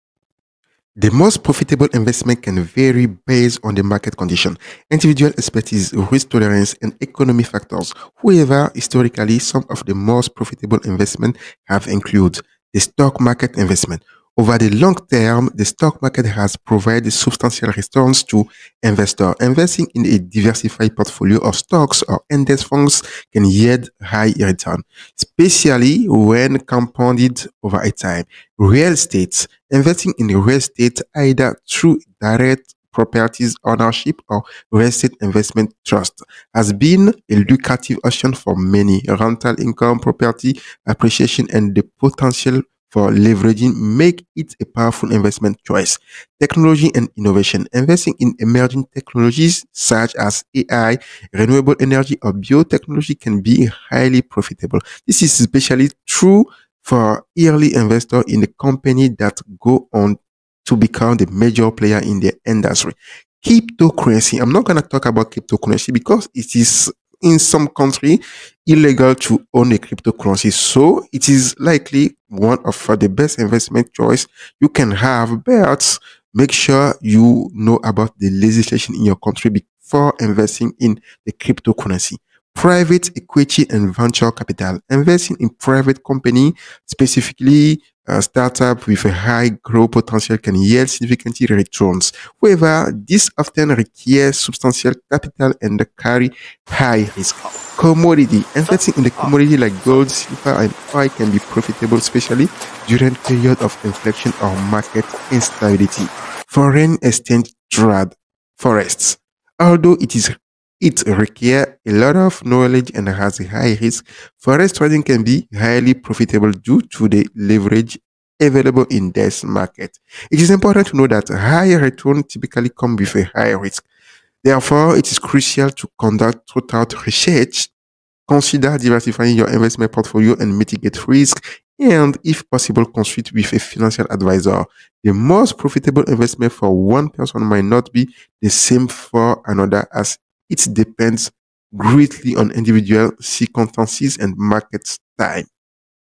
Most Profitable Investments: Strategies for Maximizing Returns
Explore the Most Profitable Investment Strategies
Choosing the right investments is crucial for building wealth, but the most profitable options can vary depending on market conditions, risk tolerance, and individual expertise. In this video, we discuss some of the historically most profitable investments and what you need to know before diving in.

Key Topics Covered:

Stock Market Investments: Learn how investing in a diversified portfolio of stocks or index funds can yield high returns over time, particularly when compounded.
Real Estate: Discover the benefits of investing in real estate, from rental income to property appreciation, and how it can be a powerful wealth-building tool.
Technology & Innovation: Explore opportunities in emerging technologies such as AI and renewable energy, and understand the potential rewards for early investors.
Private Equity & Venture Capital: Understand the high-risk, high-reward nature of investing in startups with significant growth potential.
Commodities: Consider the role of commodities like gold and silver in your portfolio, especially during periods of market instability.
Important Note on Cryptocurrency: While cryptocurrency can be a lucrative investment, it's important to be aware of the legal implications in your country before investing.

Timestamps:

00:00 Introduction: Most Profitable Investments
00:14 Stock Market Investments
00:30 Real Estate Opportunities
00:46 Investing in Technology & Innovation
01:10 Private Equity & Venture Capital
01:25 Commodities as Safe Havens
01:40 Considerations for Cryptocurrency
02:00 Final Thoughts on Investment Strategies